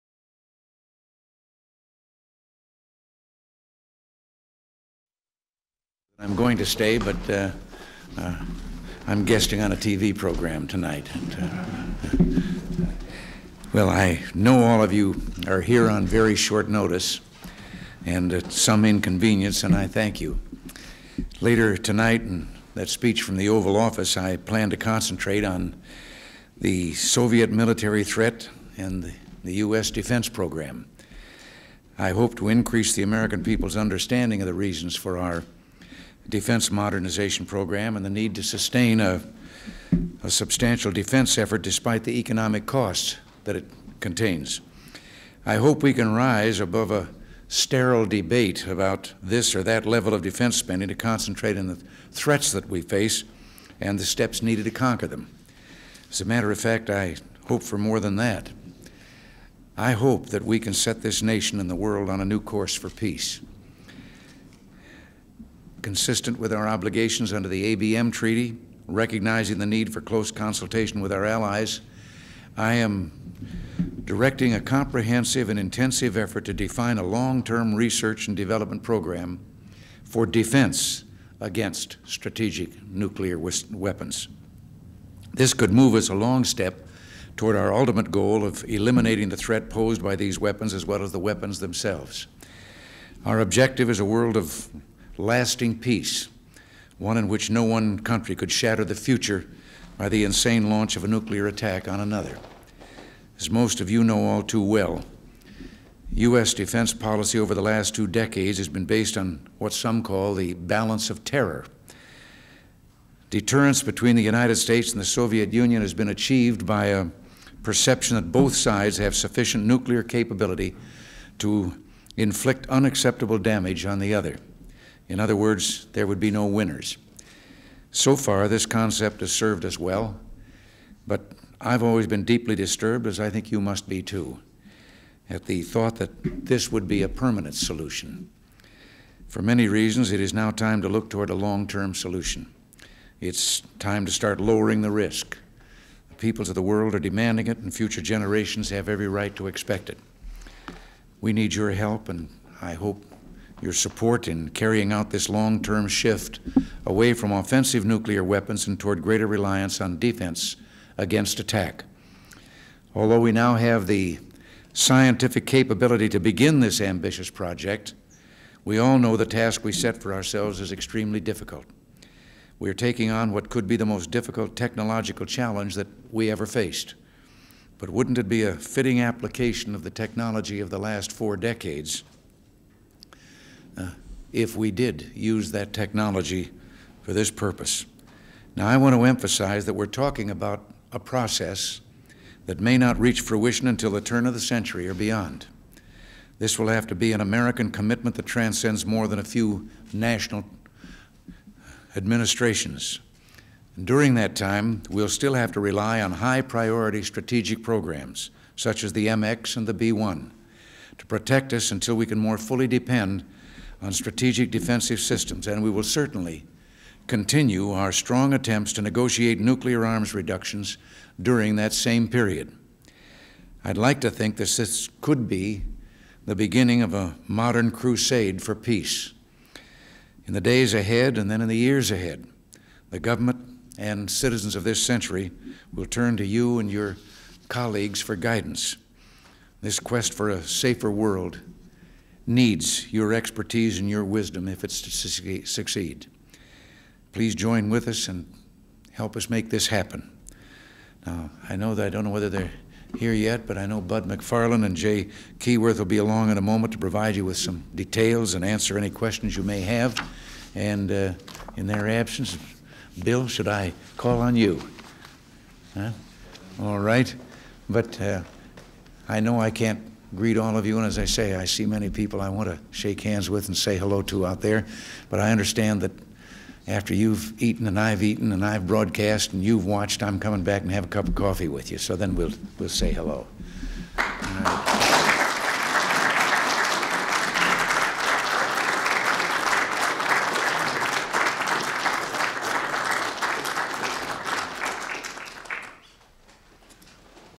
President Reagan’s Remarks on the Strategic Defense Initiative on March 23, 1983
Full Title: President Reagan’s Remarks on the Strategic Defense Initiative (SDI) to High Level Defense Officials and Staff in the Blue Room on March 23, 1983

Transcript: N/A

Production Date: 3/23/1983

Access Restriction(s):Unrestricted
Use Restriction(s):Unrestricted